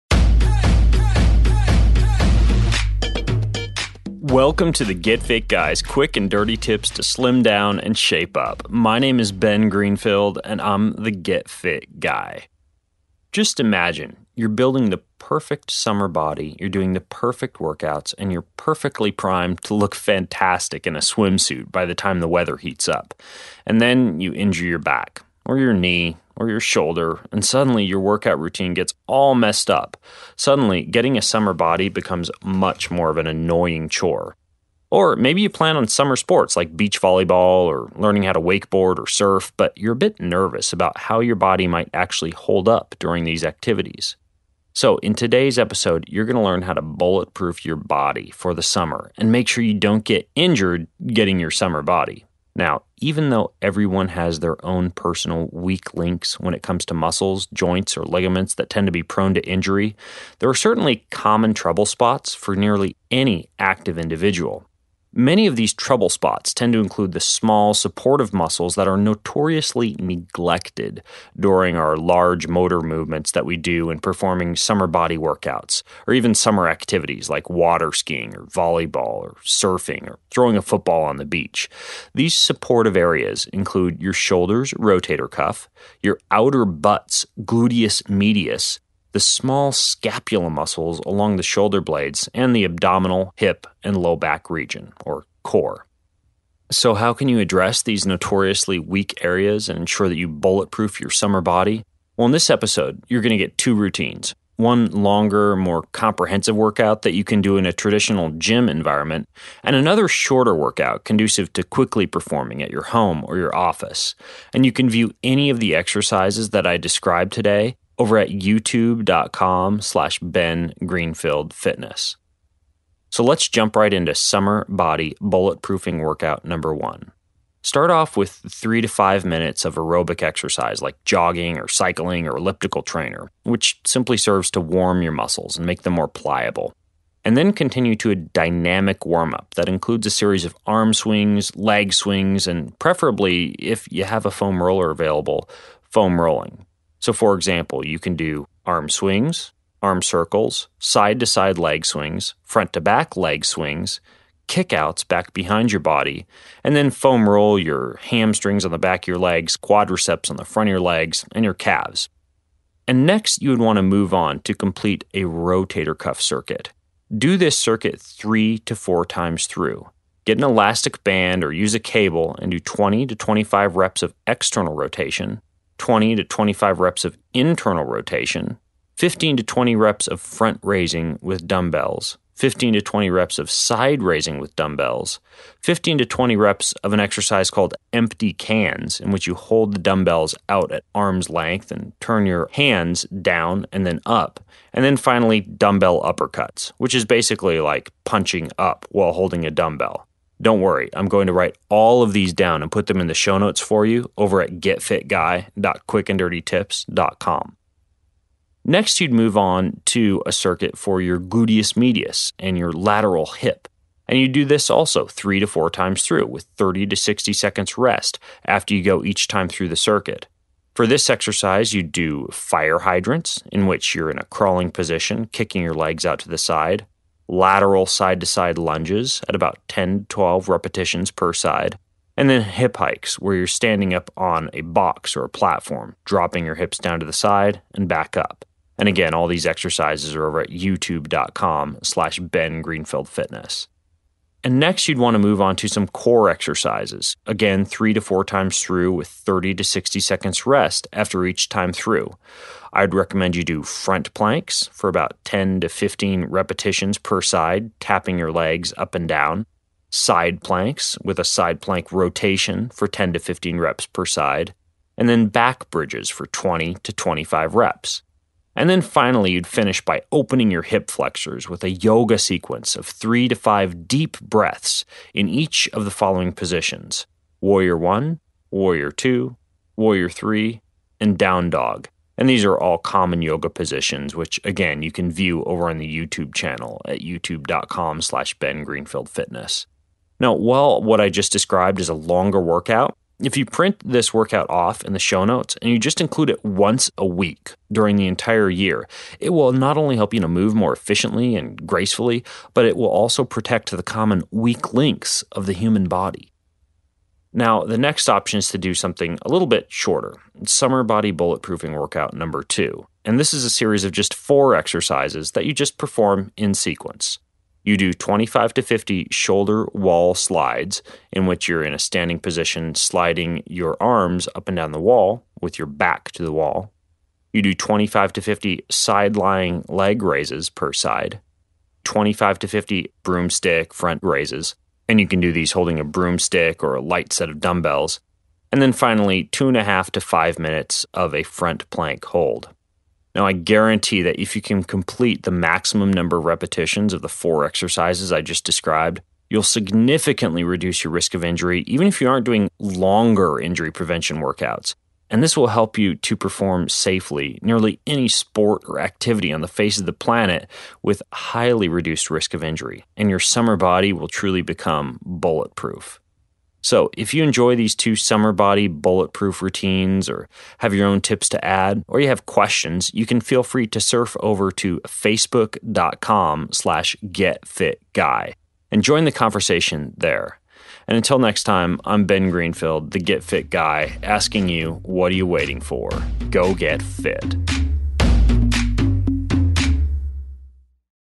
141 GFG Summer Body Injury-Proofing
Learn how to injury proof your body for the summer and make sure that you don't get injured getting your summer body.